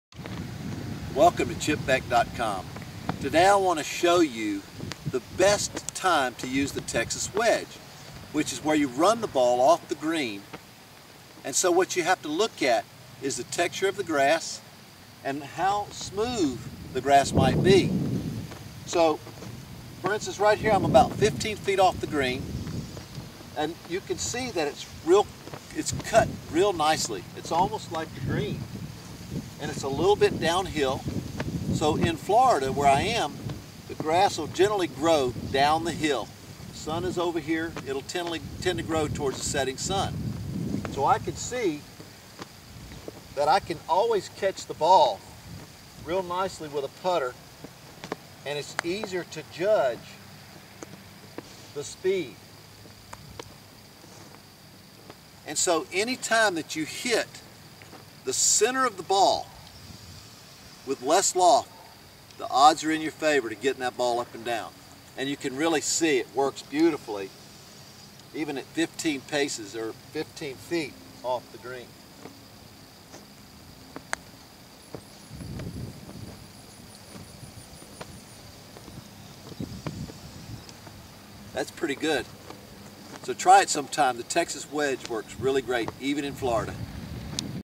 Putting using Texas Wedge
Here I show you when to use the Texas Wedge off the green.  I show you how to read the greens and try something new.  It's good to have different putting options when your faced with unique situations.  Hope these tips help you become a better putter and player.  Enjoy your game and stay positive!   Chip